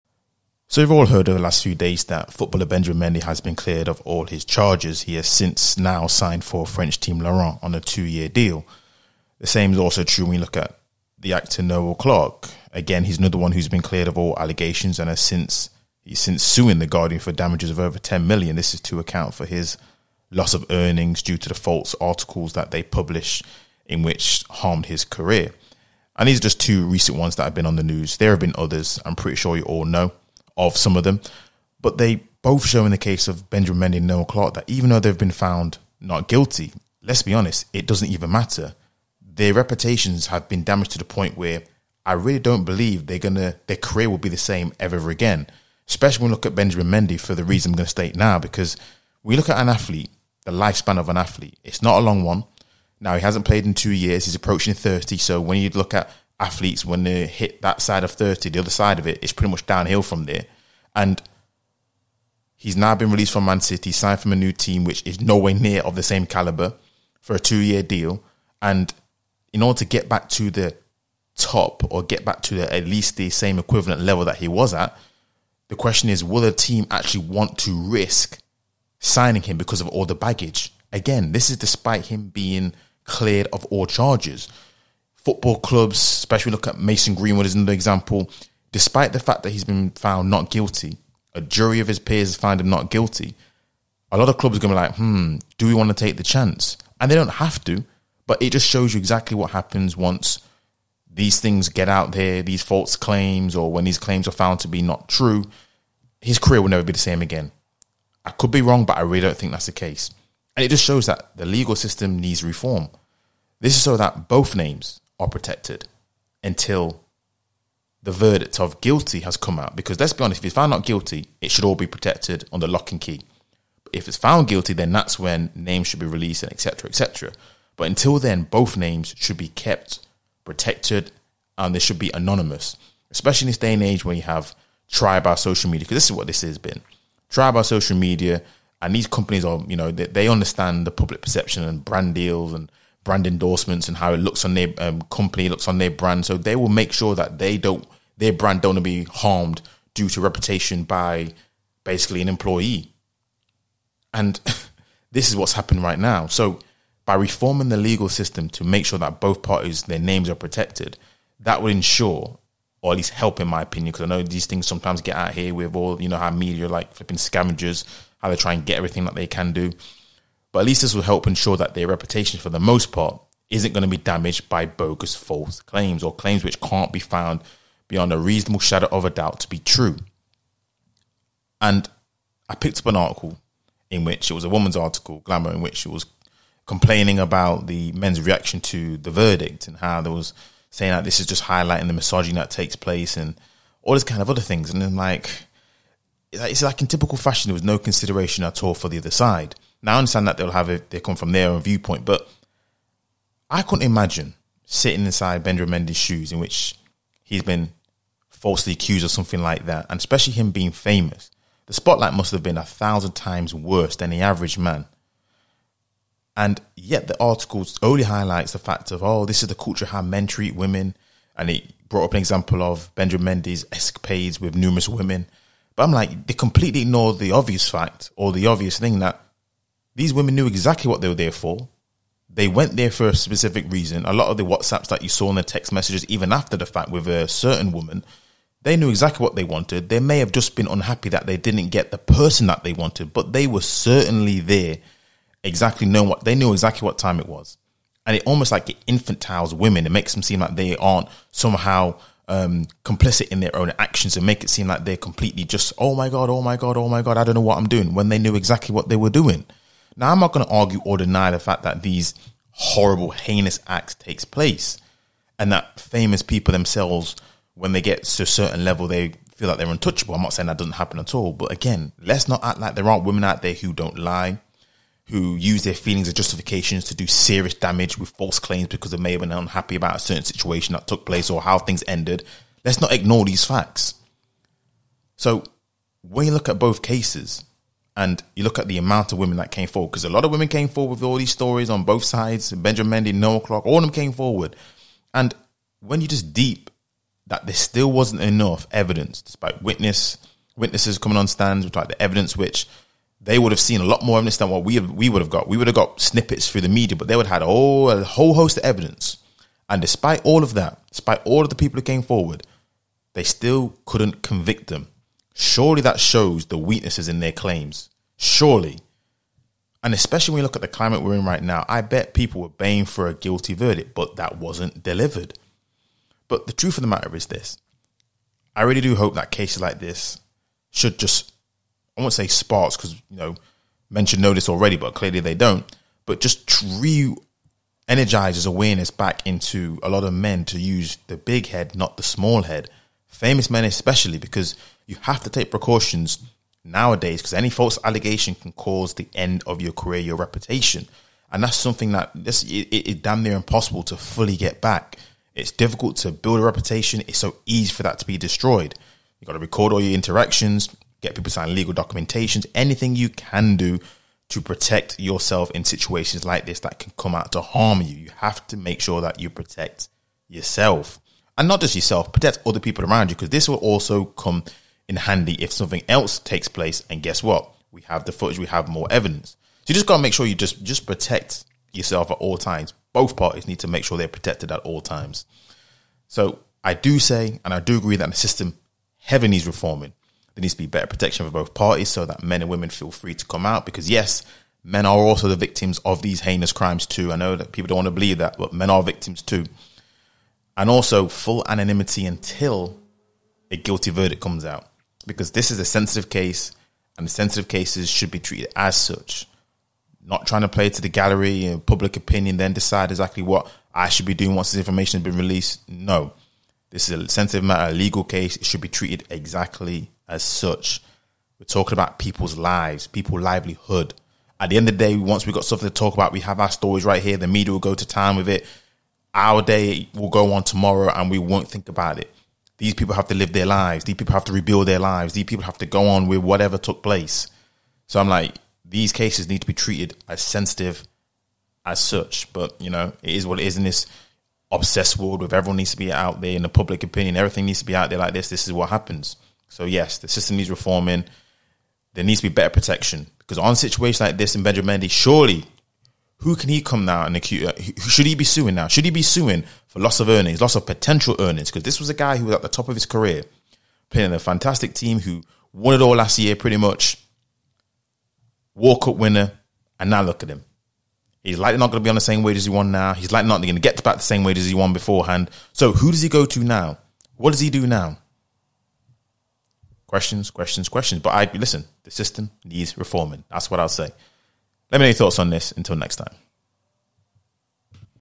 Benjamin Mendy & Noel Clarke Case Shows That Legal Reform Is Needed
Footballer Benjamin Mendy and Actor Noel Clarke has since has all charges dropped in the last few days.

As they try to rebuild their career, surely this shows that legal reform is needed?